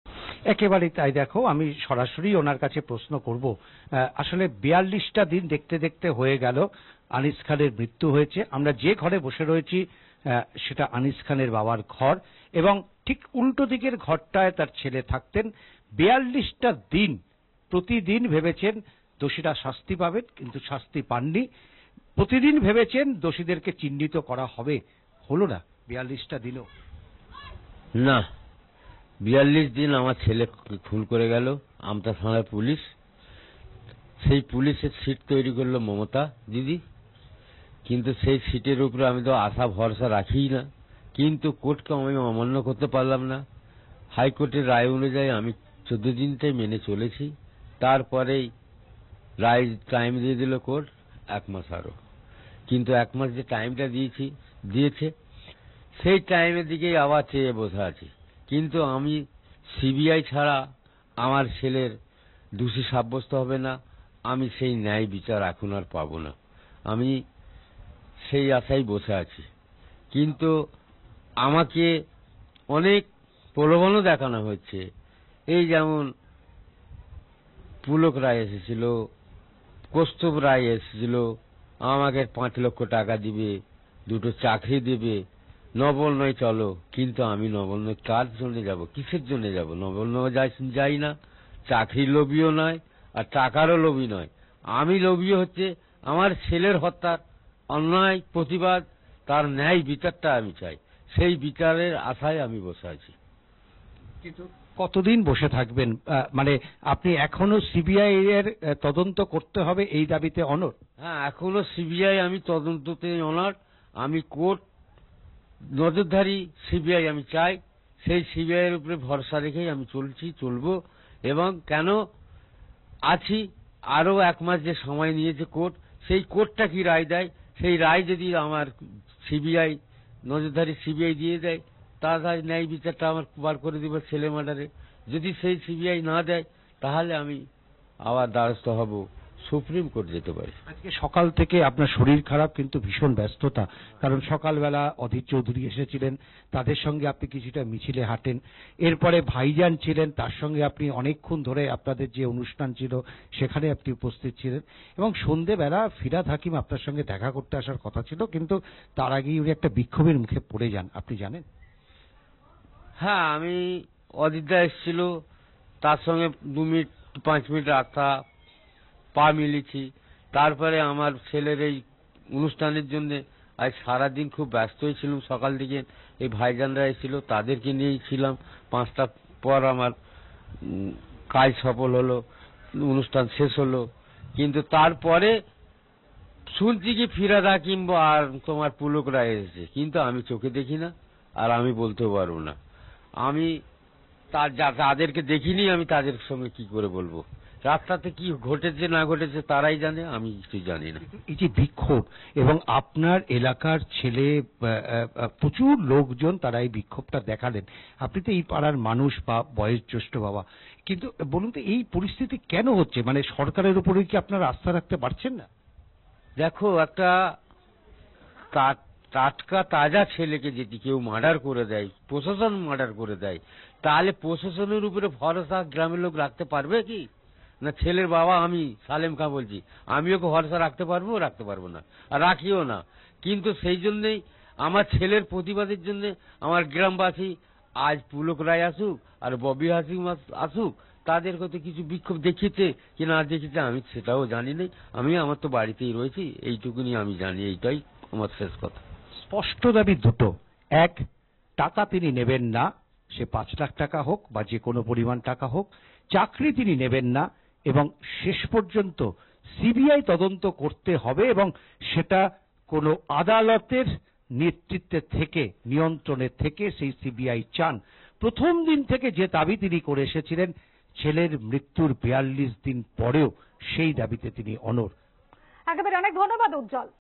Anish Khan Death: 'আমাকেও এসেছিল কেনার জন্য, বলে দিয়েছি টাকা-চাকরির দরকার নেই', জানালেন আনিসের বাবা
'আমাকেও এসেছিল কেনার জন্য, আমি কেনার লোক নই, আমি বলে দিয়েছি টাকা, চাকরির দরকার নেই', প্রতিক্রিয়া প্রয়াত ছাত্র নেতা আনিস খানের (Anish Khan) বাবার।